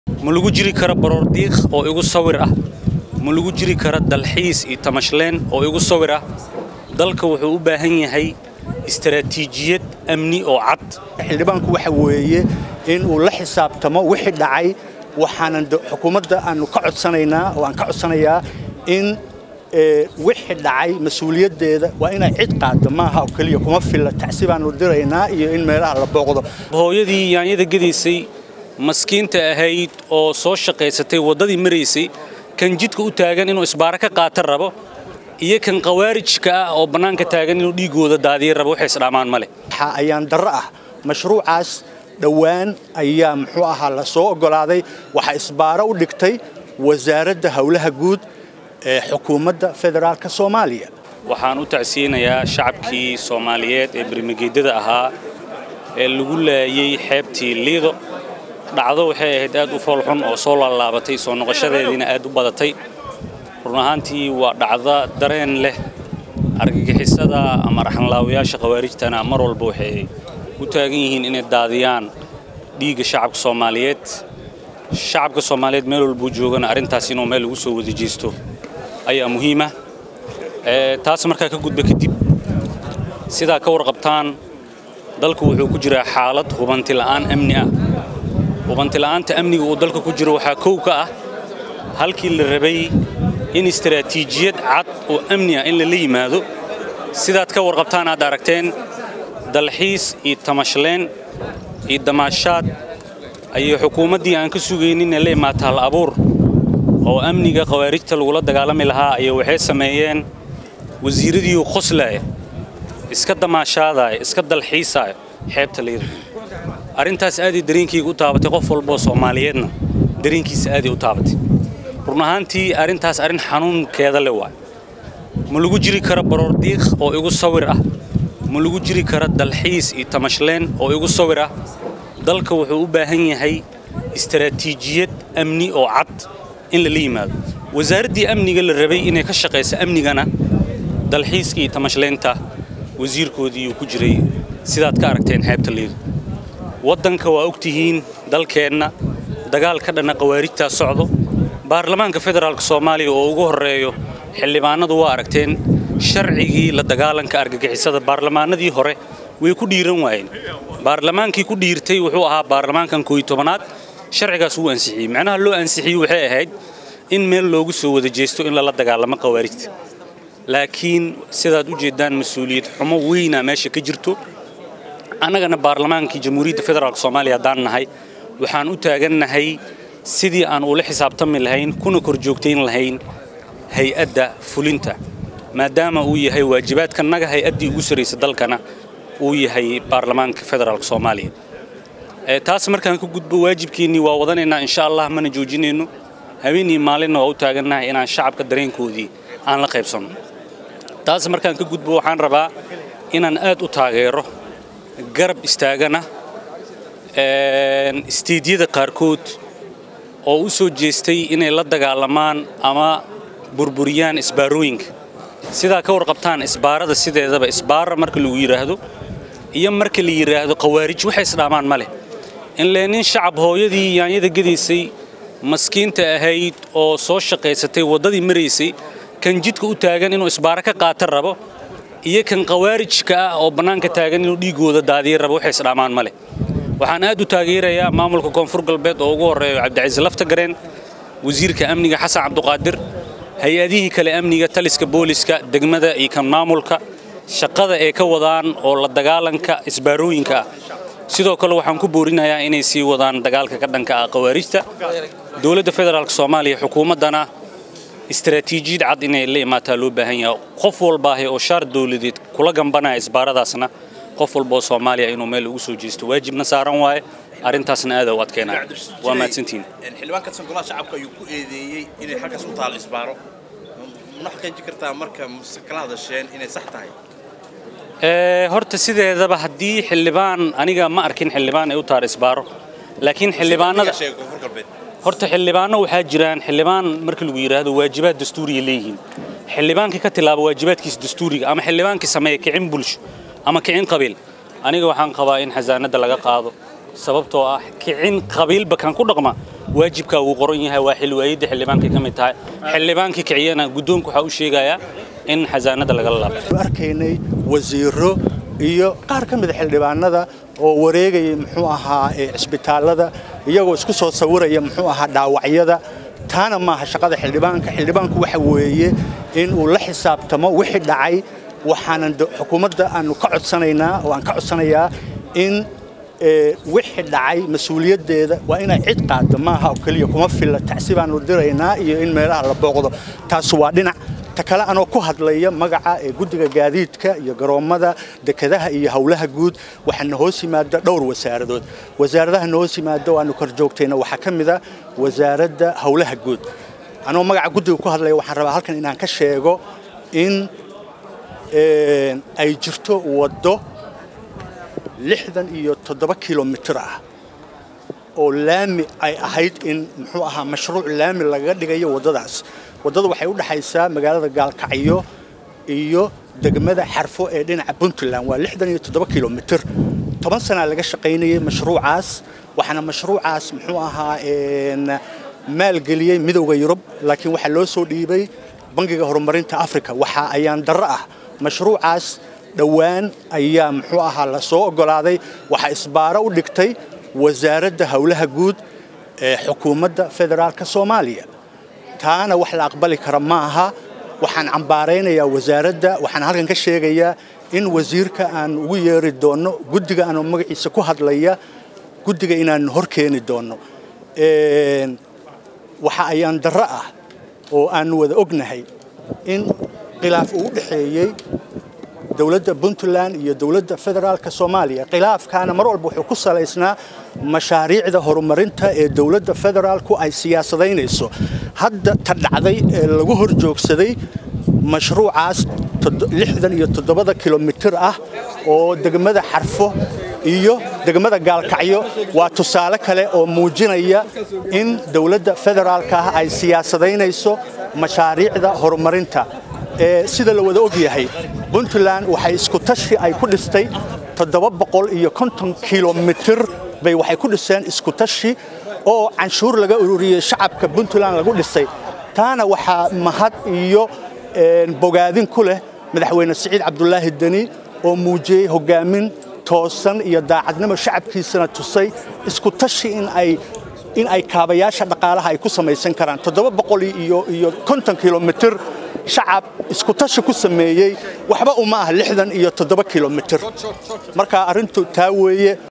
"Ma lagu jiri karo baroordiiq igu sawir ah, dalka wuxuu u baahan yahay istaraatiijiyad amni oo cad"
Xildhibaan Cabdirashiid Jili iyo Xildhibaan Wacays, BFS, oo dhaliilo soo jeediyay xukuumadda Soomaaliya.